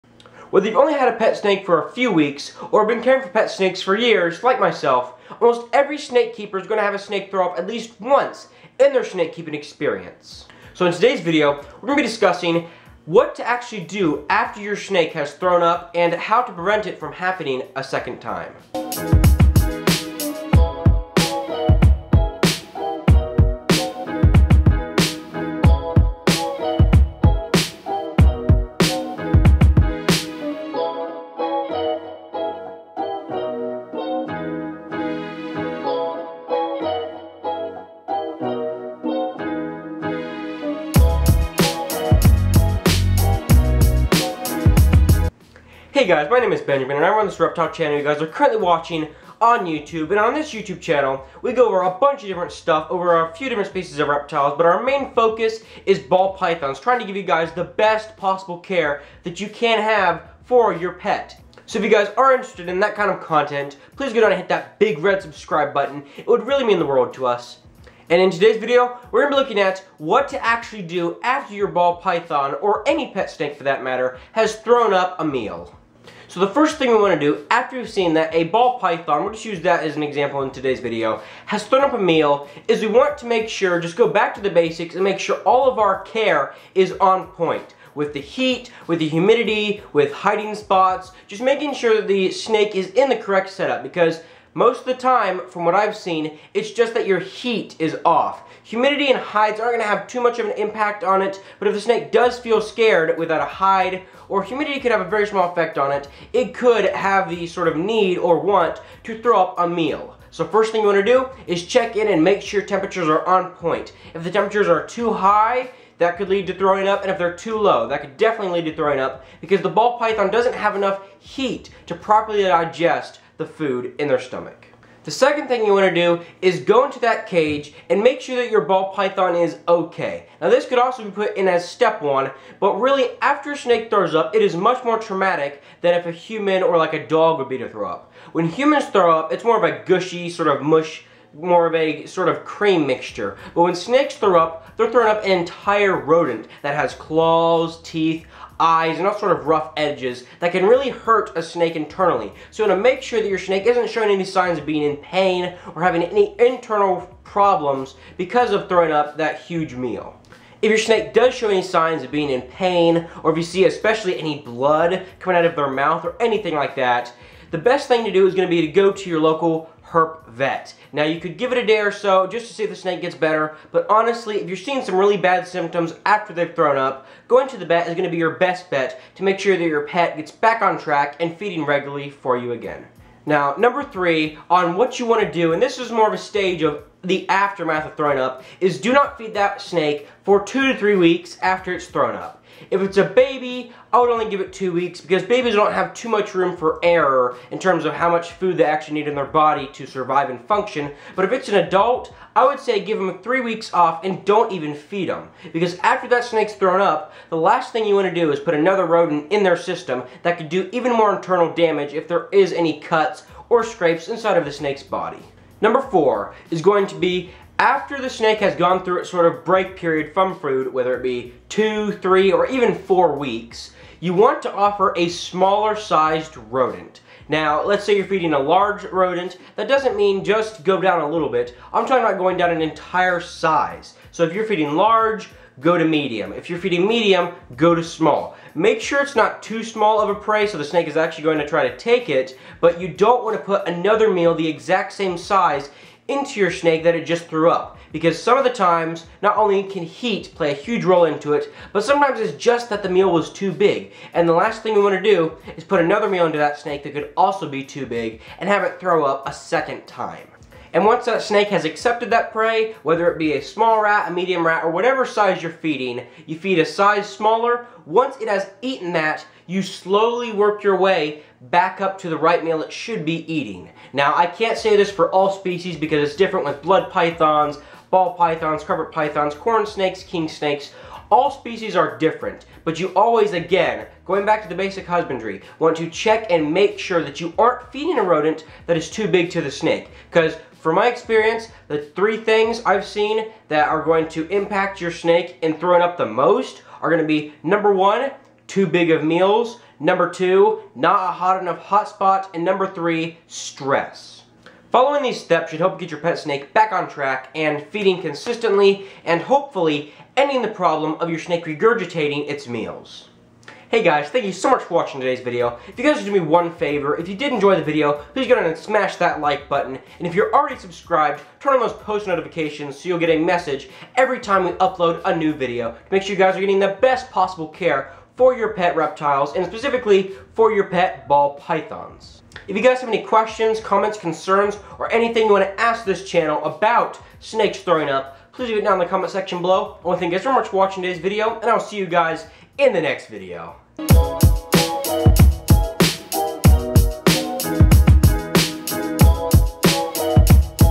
My Pet Snake Threw Up!!! What To Do? - Benjamin's Exotics
Hey Guys. In today's video, we answer a question I got from a Subscriber, "My Pet Snake Threw Up!!! What To Do?". Thankfully snake regurgitation is rare, but unfortunately it barley ever gets discussed in the Reptile Community.